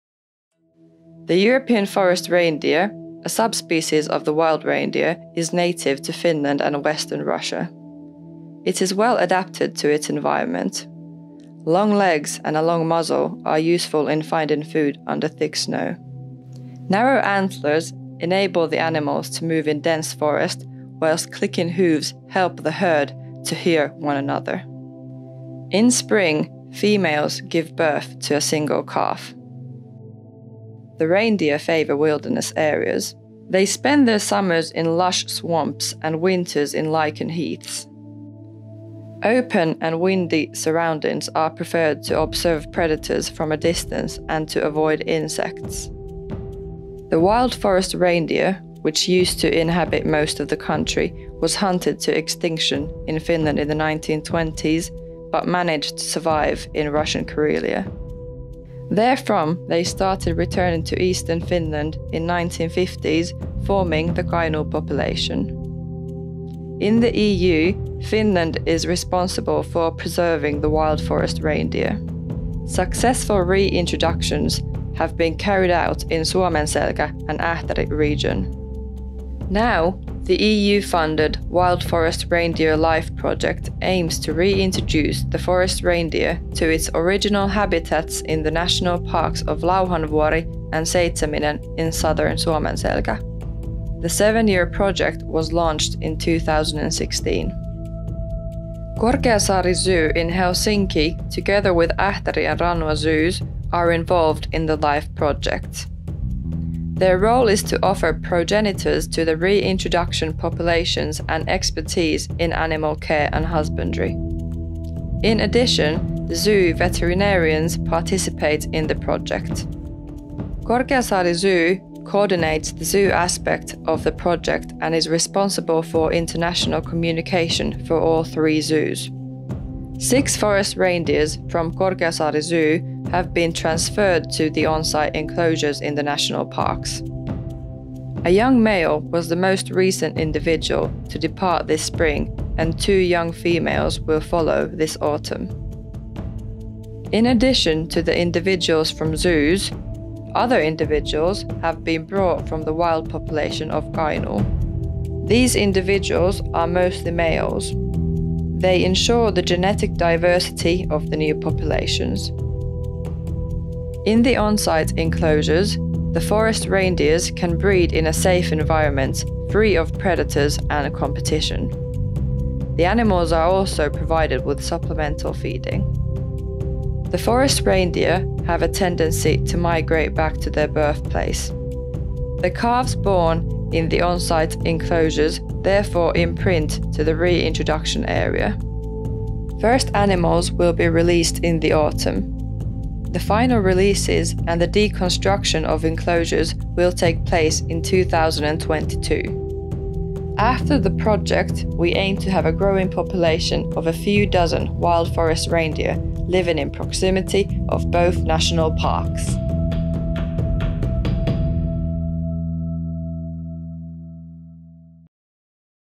WildForestReindeerLIFE (MetsäpeuraLIFE-hankkeen englanninkielinen esittely)
European forest reindeer (Rangifer tarandus fennicus) roamed almost everywhere in Finland until it was hunted to extinction in the 1900s. Luckily a population was left in the Russian Karelia, and the species walked across the border to Kainuu in 1950s. In the 1970s the forest reindeers were reintroduced back to the species original habitats in Suomenselkä, and the reintroductions continue with WildForestReindeerLIFE project (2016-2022) funded by EU.

Production: Annika Sorjonen / Korkeasaari Zoo (2019)
Videographers: Sami Saarela, Henriks Sundén, Pasi Hornamo
Script: Elmo Miettinen
Voiceover: Aliisa Peters